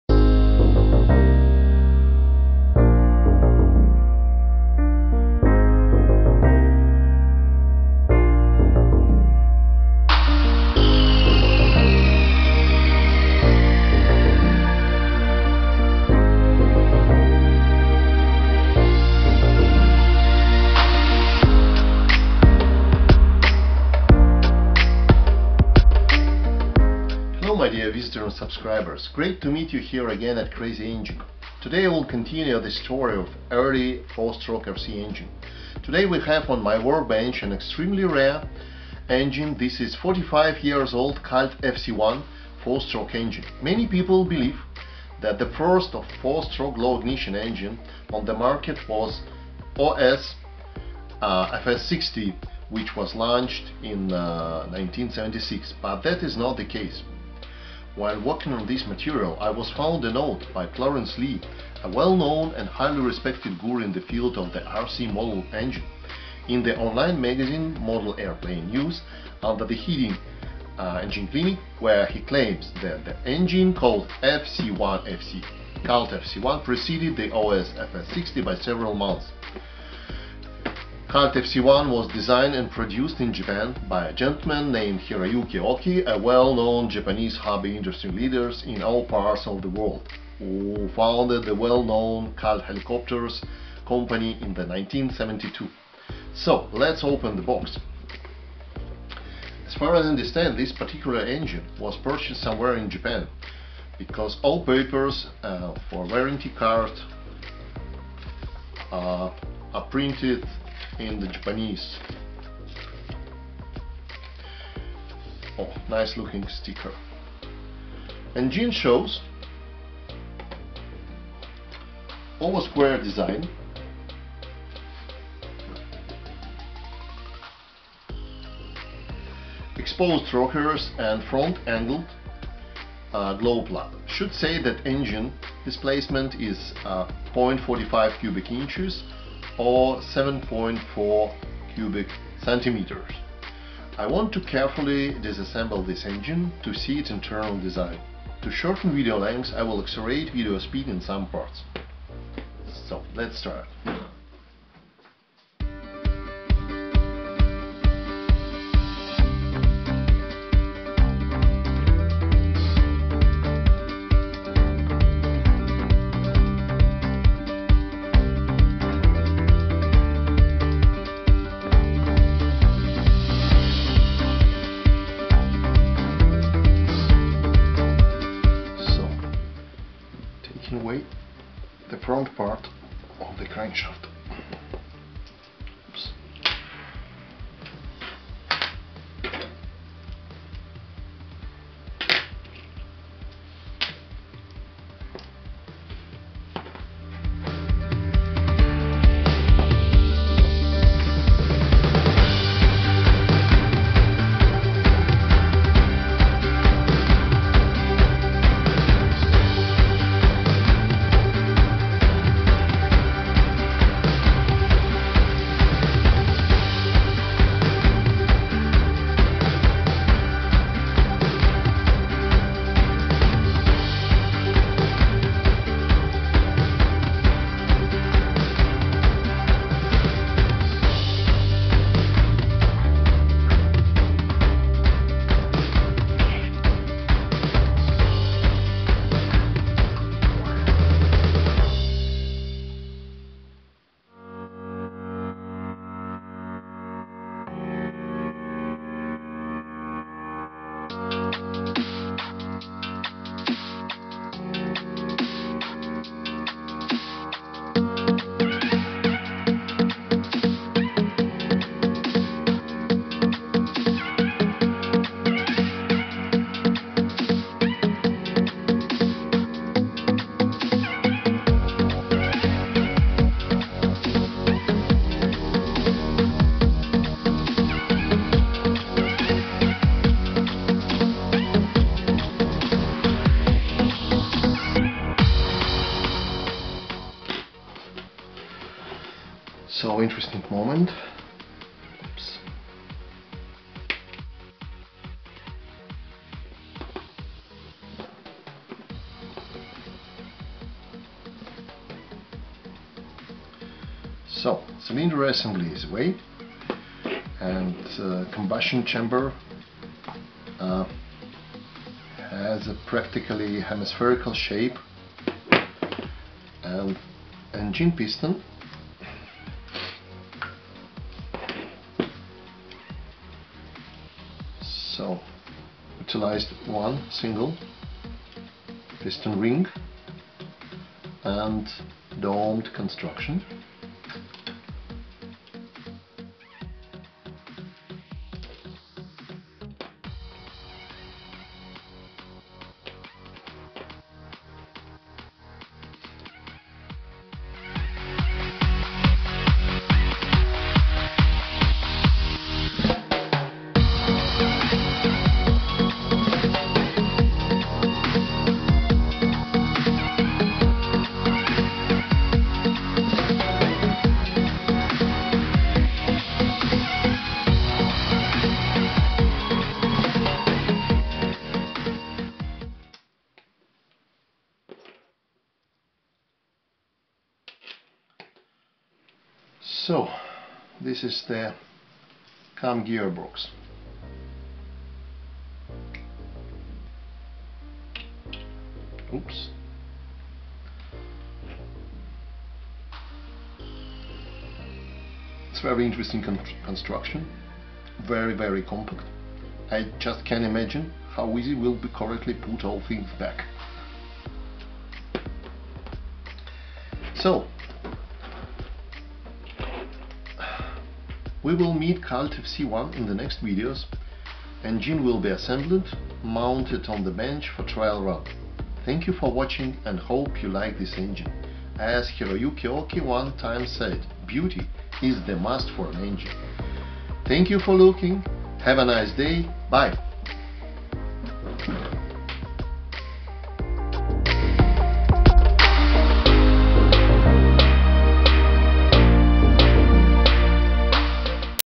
Kalt FC 1 mega rare engine history
This video is dedicated to the Kalt FC 1,  to one of the rarest and collectible 4 stroke engines, designed  and developed by Mr.Hiroyuki Oki, founder of the world wide known Kalt Helicopters Company.
Engine, called Kalt FC 1, was launched on the market several months earlier, than famous OS FS 60, and probably is the first mass produced 4 stroke RC engine. Engine shows amazingly great design and build quality.
This video includes some historical facts about engine, as, well, as partial disassembling, to learn engine's internal design features.
I'm personally was very exited when working with this piece of real Art of precision mechanics and engineering.
You're kindly invited to the world of CrazyEngines, let's look, learn and share the experience !